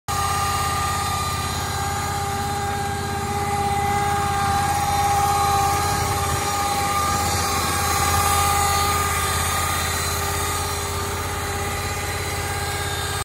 GovDeals: John Deere 1200 Hydro Bunker Rake
John Deere 1200 Hydro Bunker Rake sold by Harrisburg, NC on GovDeals.com